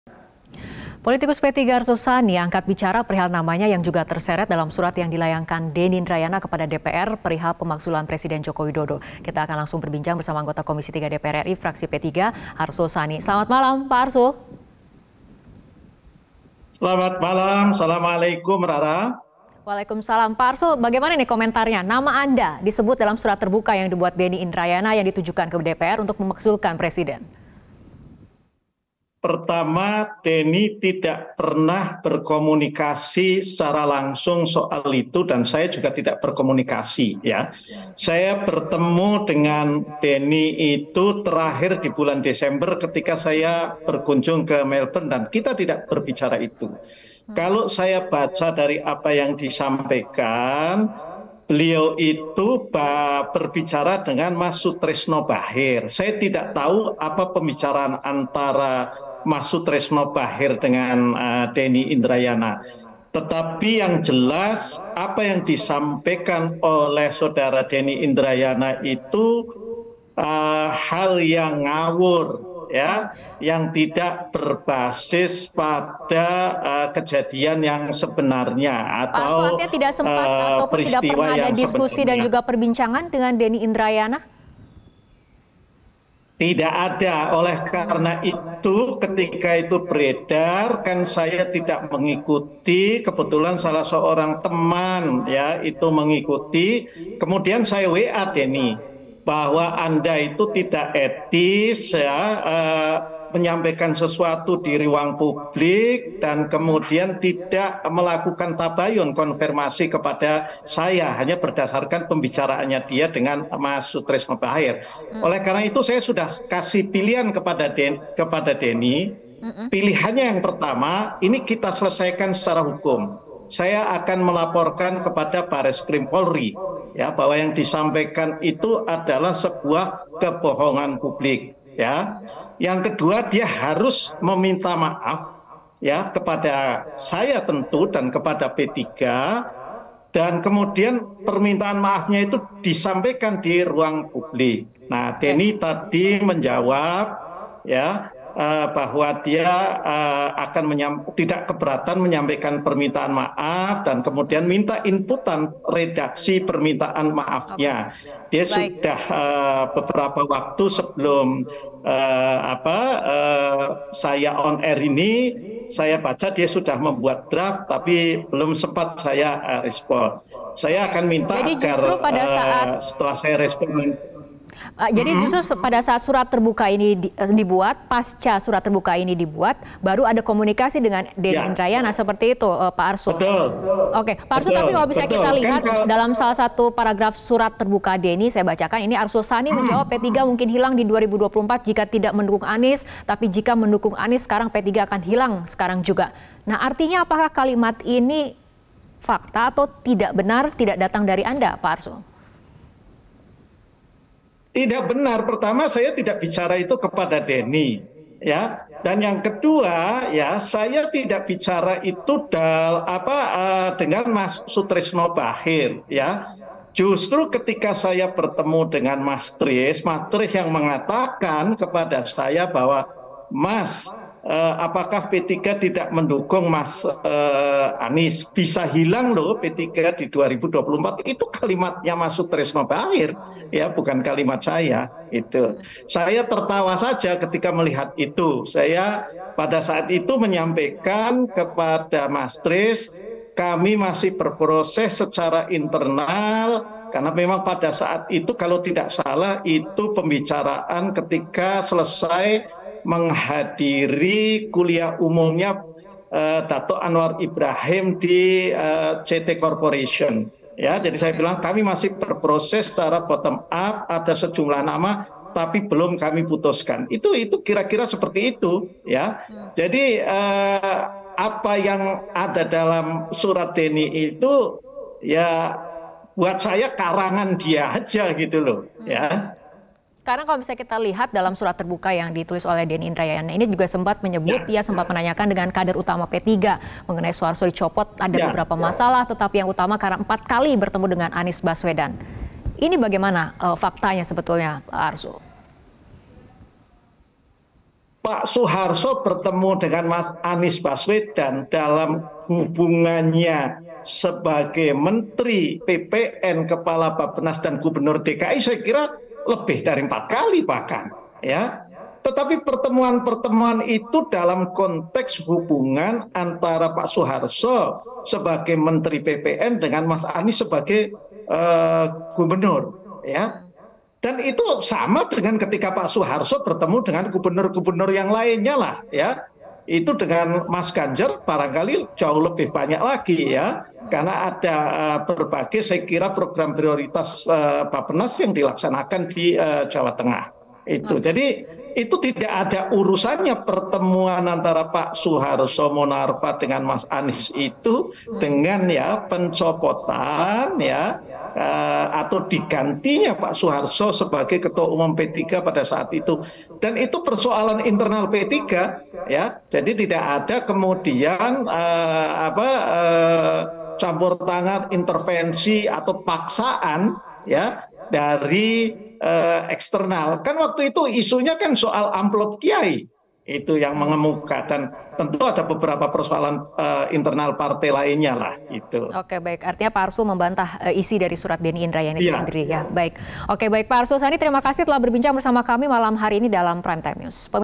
Denny Usul Presiden Dimakzul, Arsul 'Tersundul'
MetroTV, Politikus fraksi PPP, Arsul Sani angkat bicara mengenai namanya yang juga ikut terseret dalam surat yang dilayangkan Denny Indrayana kepada DPR perihal pemakzulan presiden Joko Widodo.